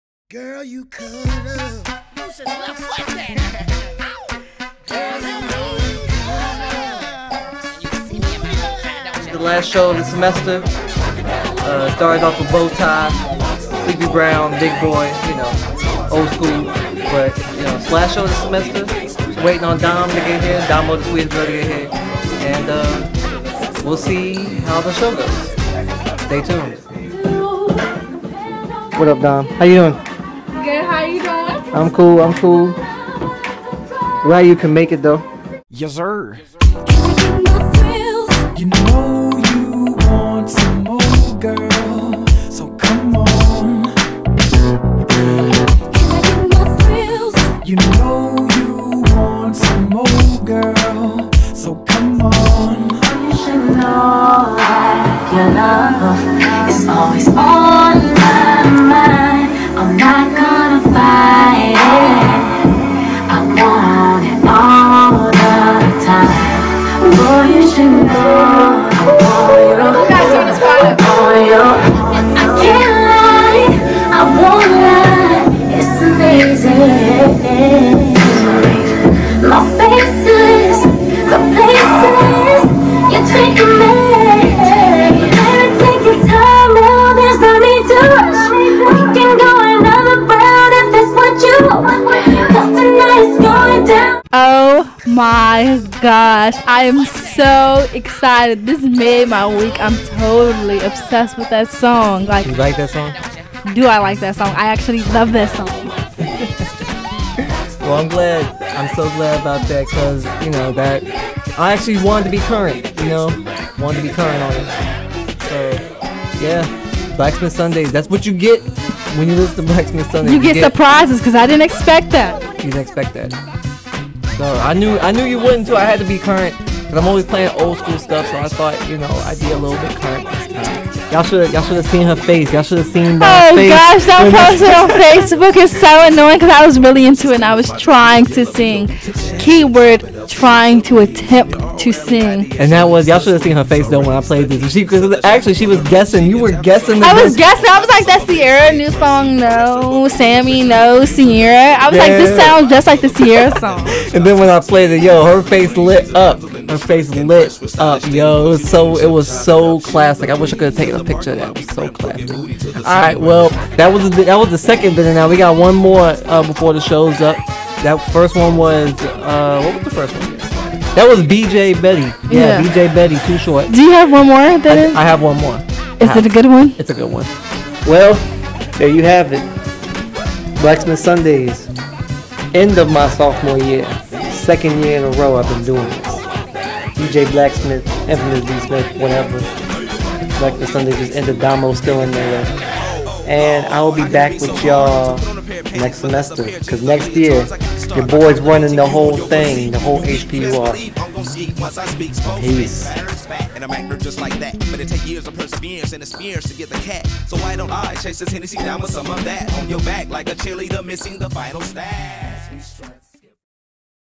BlackSmith Sundays Documentary #2
Last show of the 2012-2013 school year for DJ BlackSmith & Domo The Sweetest Girl!